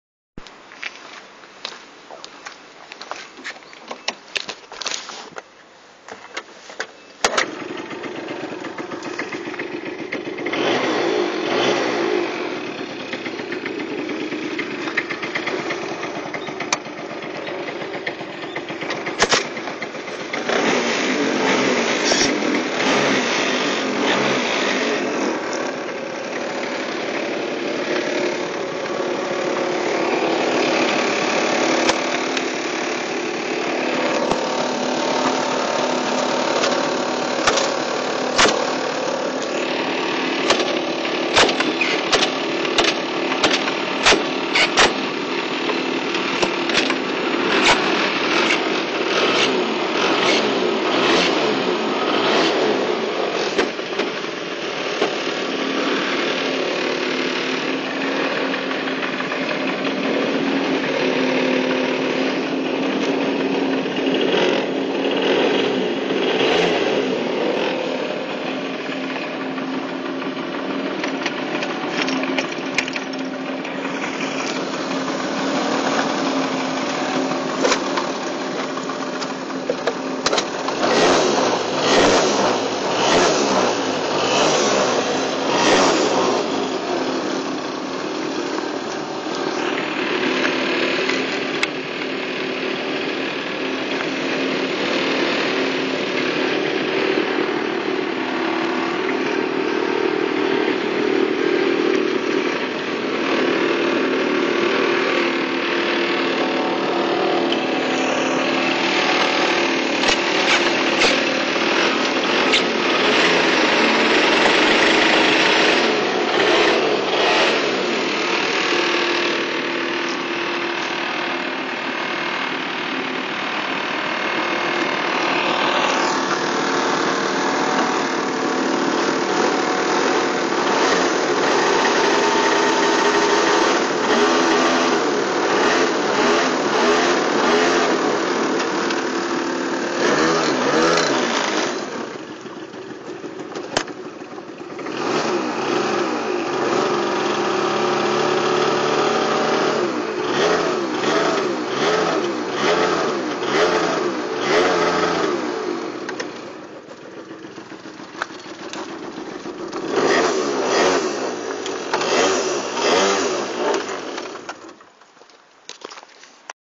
Export From japan GYRO UP USED JAPANESE MOTORCYCLES SCOOTER GYRO-UP GYRO-X
USED Scooter Export from osaka japan.
Sold to lebanon.Thanks.

export from osaka japan